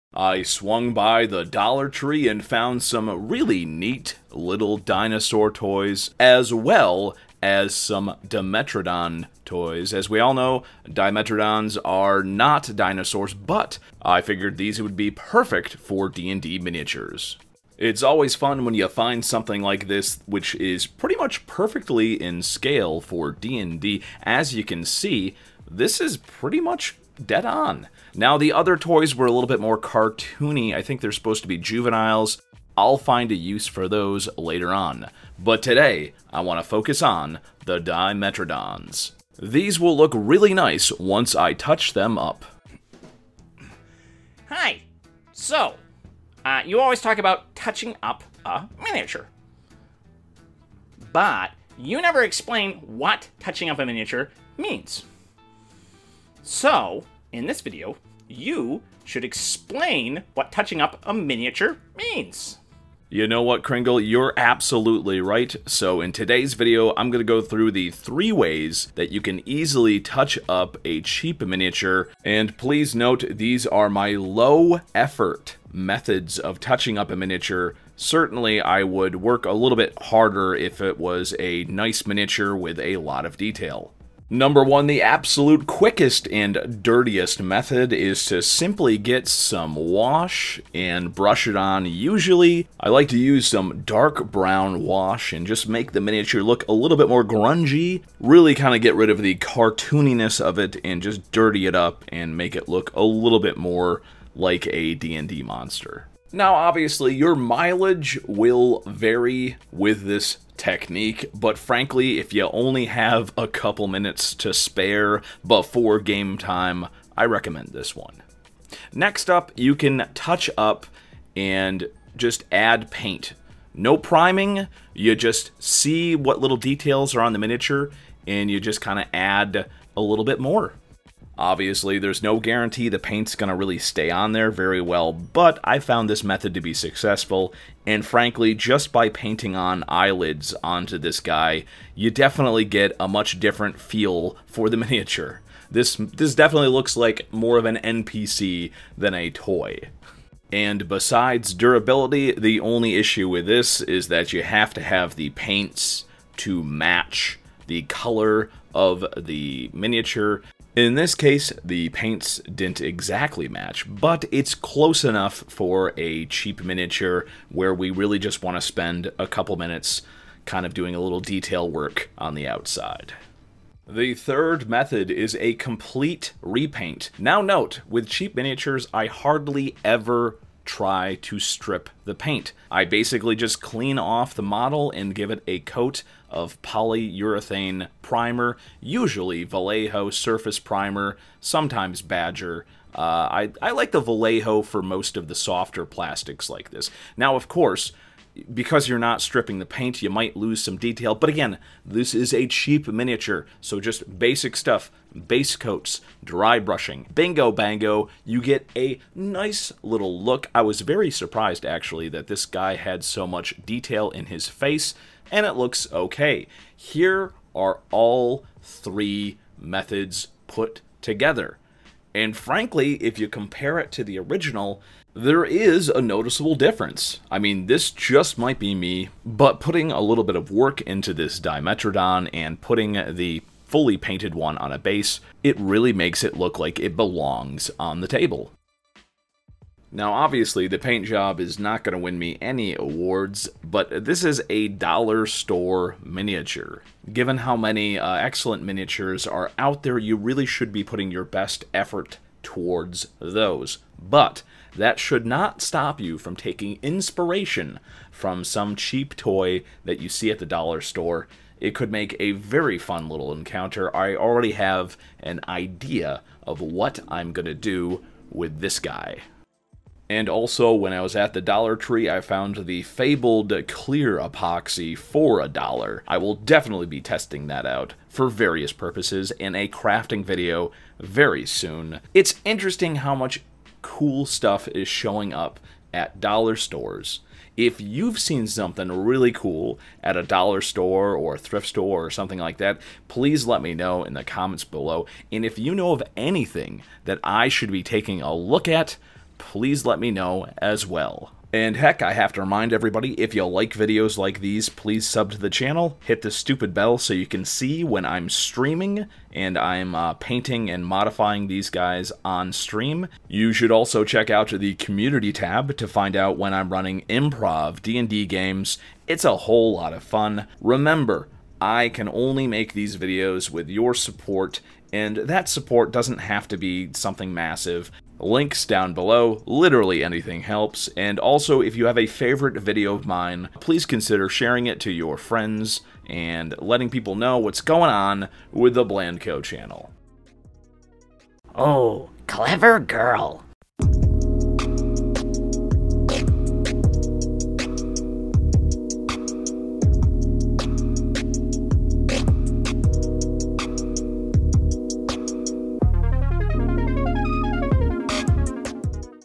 3 Methods to Improve Dollar Store Finds for D&D - Dollar Store Dimetrodon
The best and quickest ways to improve inexpensive Dollar Tree miniatures for D&D.